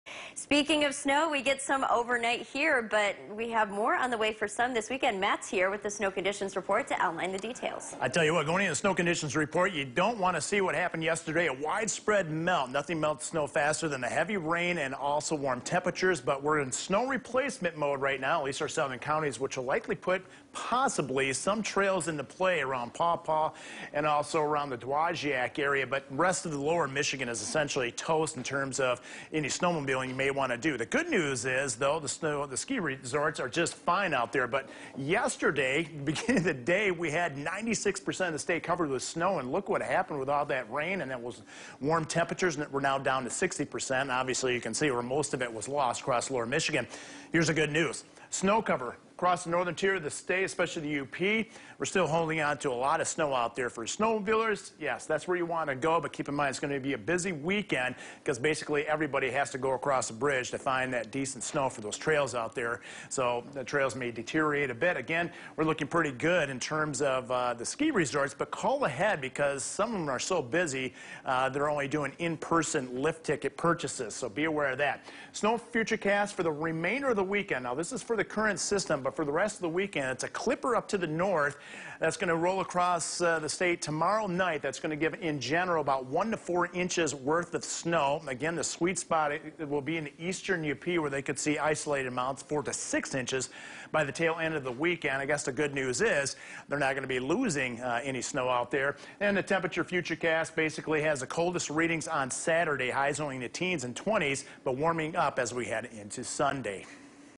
Snow Conditions Report: Feb. 17, 2022
Presidents Day weekend typically represents an extra long weekend for families to have fun in the snow, but while Wednesday’s rain and warmth may have felt good, it was not good for winter sports. (Feb. 17, 2022)